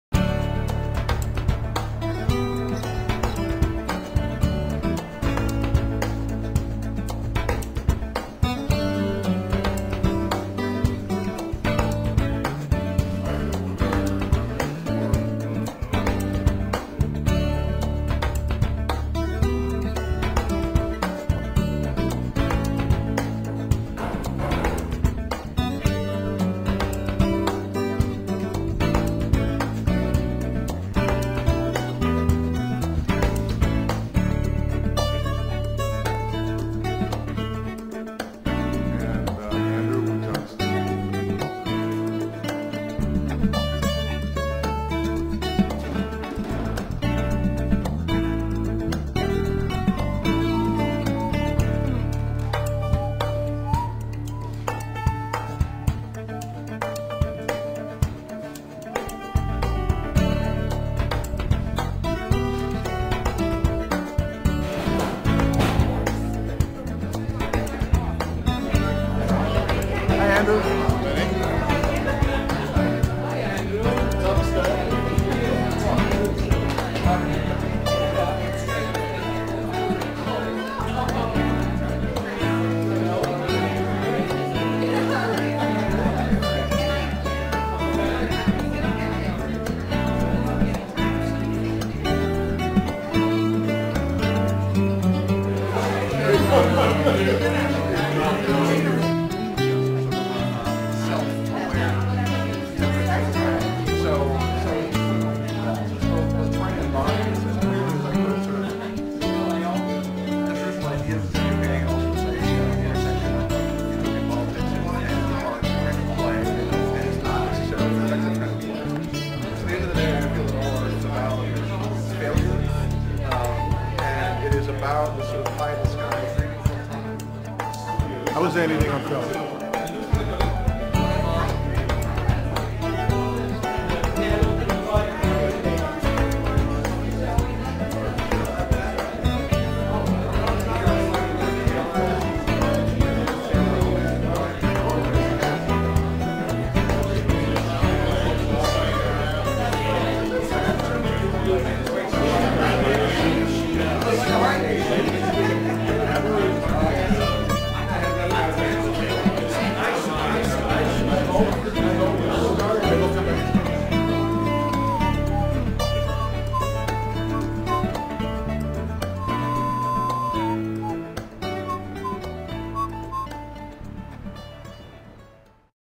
"Color Invitations" Gallery Opening at R Street Gallery, DC
Opening reception for the "Color Invitations" exhibition at Washington, DC's R Street Gallery. Features work by Maggie Michael, Amy Lin, Andrew Wodzianski, Jeffry Cudlin, John Blee, Steve Lapin and the curator, F. Lennox Campello.